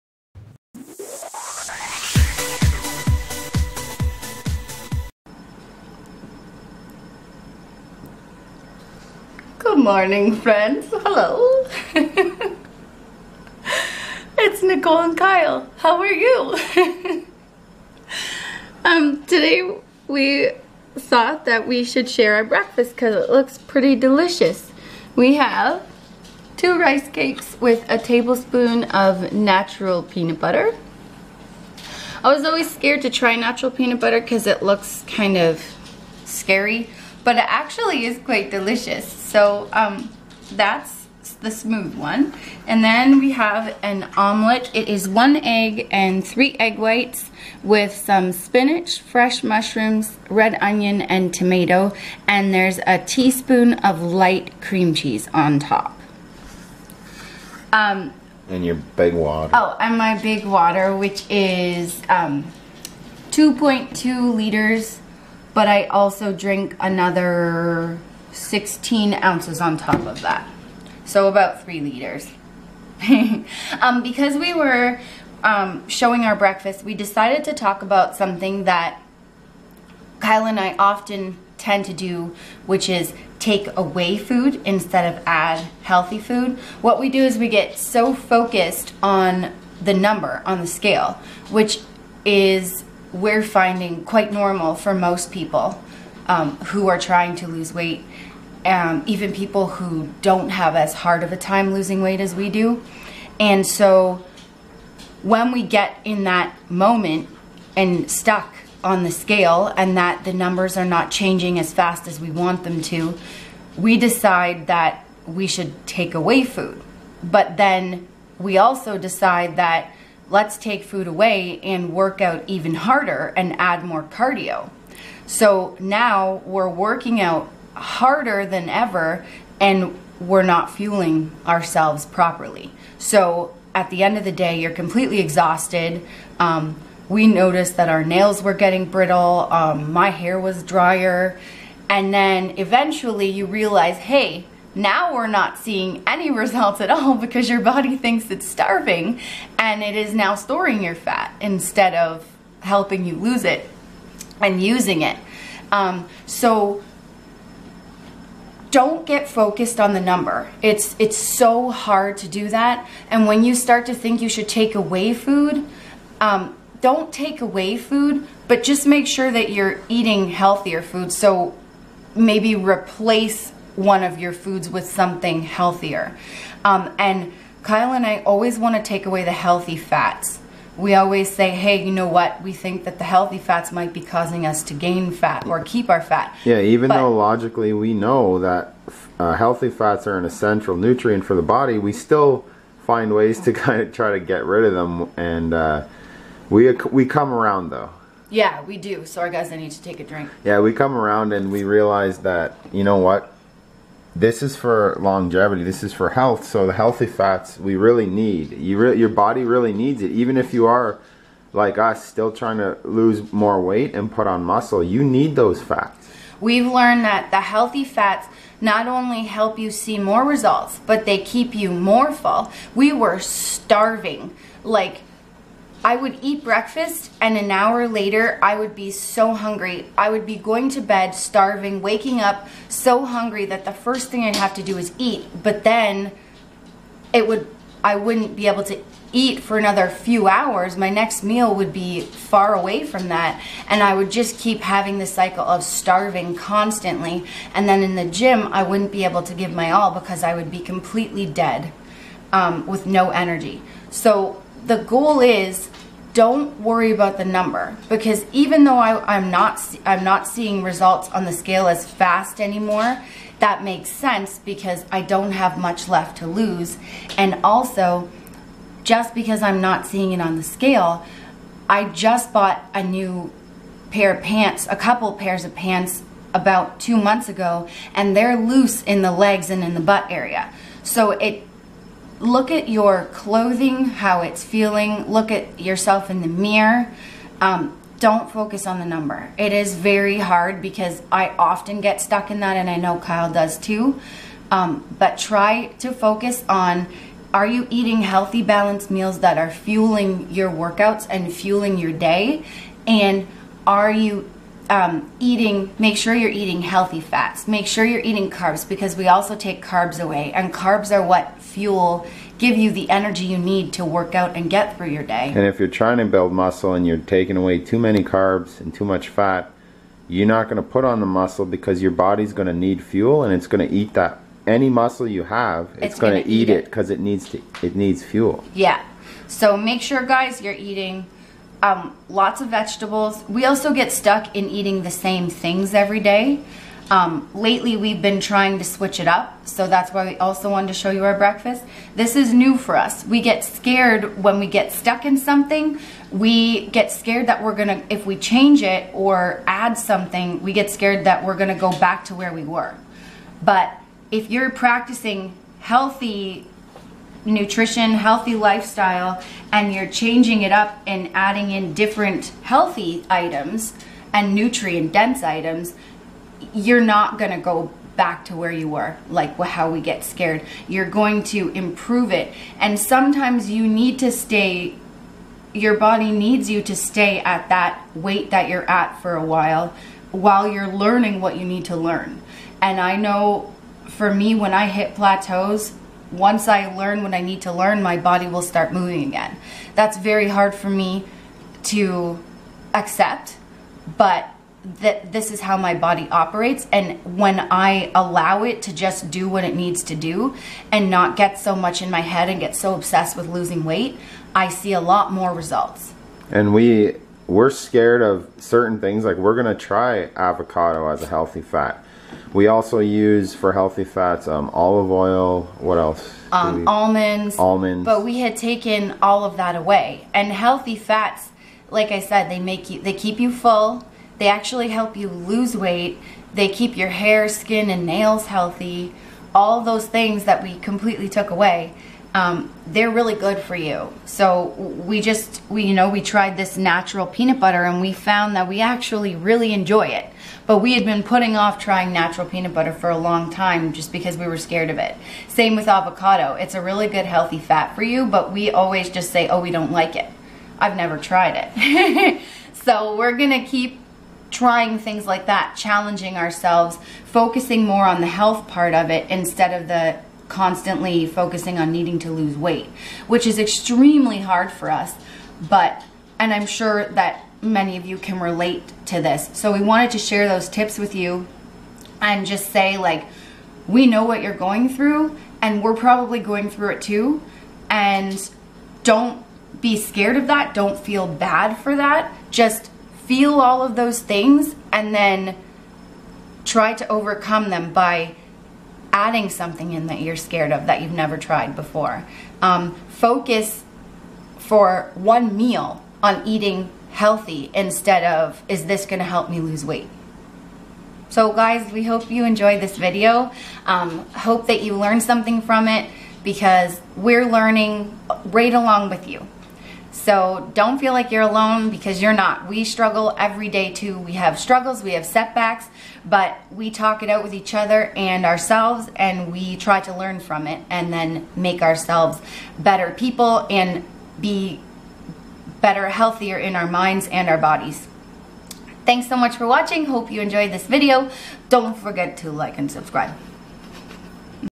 FAT TO FIT: STARVATION DIETS | Nicole Collet
Hi Guys!  Today I wanted to talk to you about my weight loss struggles along my fitness journey.  More specifically the problems that occur when we try and remove too much food from our diets